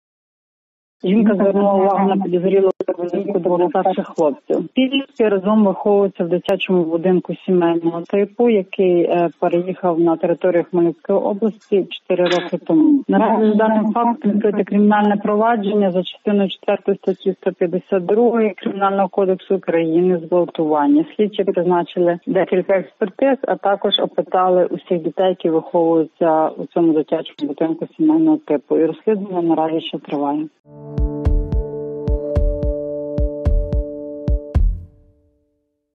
Поліція з’ясовує обставини зґвалтування двох 11-літніх хлопчиків 14-літніми підлітками
Обставини зґвалтування двох 11-річних хлопців, до якого, за попередньою інформацією, можуть бути причетні двоє 14-річних підлітків, з’ясовують слідчі Летичівського відділу нацполіції в Хмельницькій області.  Про це повідомила прес-секретар головного управління нацполіції області Інна Глега. За її словами, з відповідною заявою про злочин до органів поліції звернулася опікунка усіх чотирьох дітей.